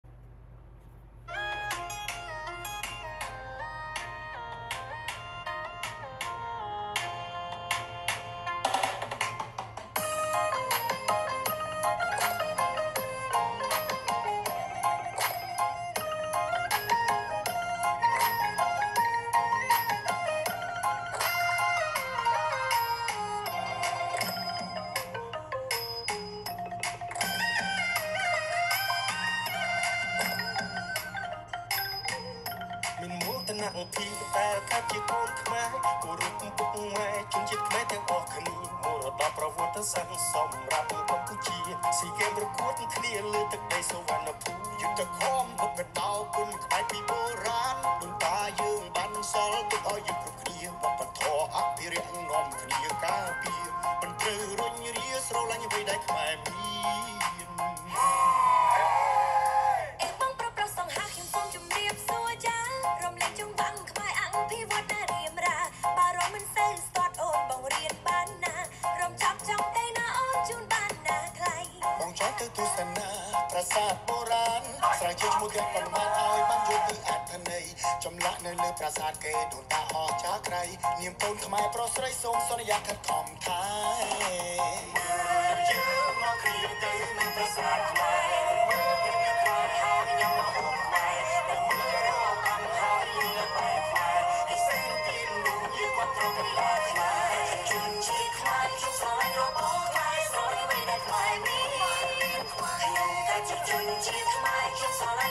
Khmer Culture display at Phoenix, Arizona. 4725 E. Mayo Blvd.Phoenix, AZ 85050
Don't miss this extraordinary opportunity to reconnect with your roots and ignite your Khmer pride. Visit the Khmer Culture display at MIM today and unlock the wonders of our extraordinary legacy!"